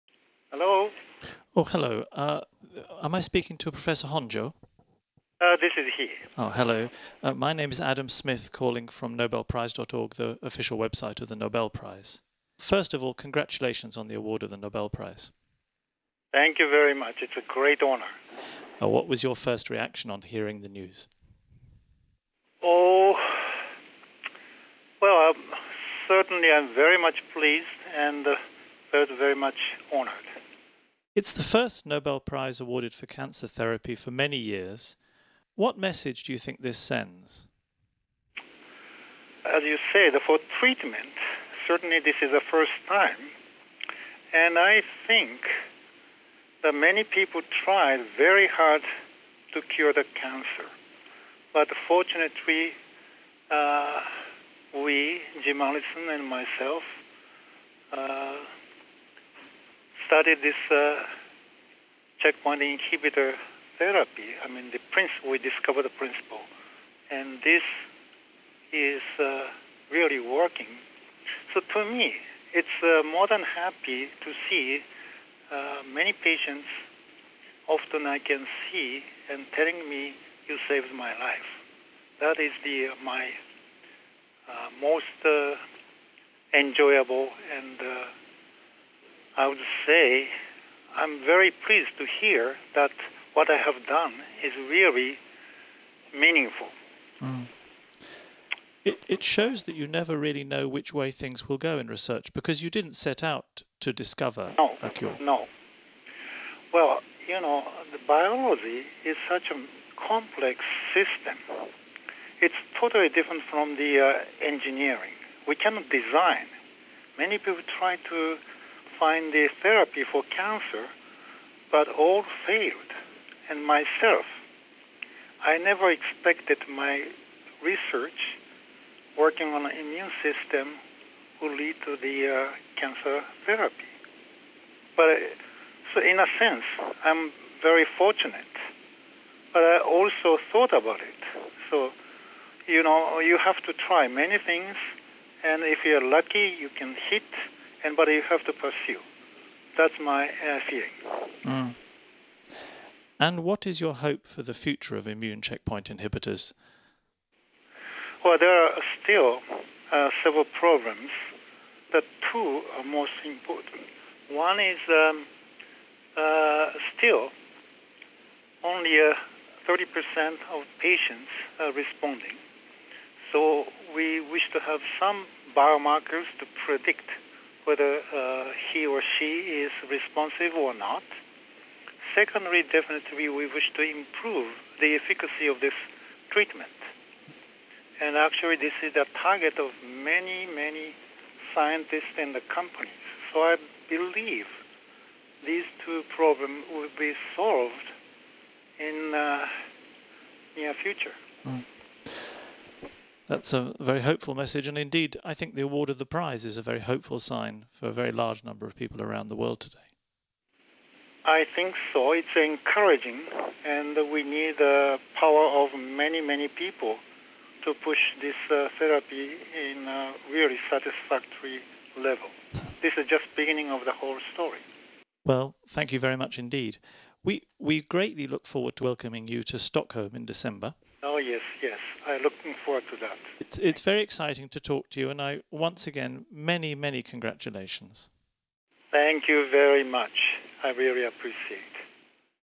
"I'm very pleased to hear that what I've done is really meaningful." Interview with Tasuku Honjo
Telephone interview with Tasuku Honjo following the announcement of the 2018 Nobel Prize in Physiology or Medicine, 1 October 2018. The interviewer is Nobelprize.org's Adam Smith.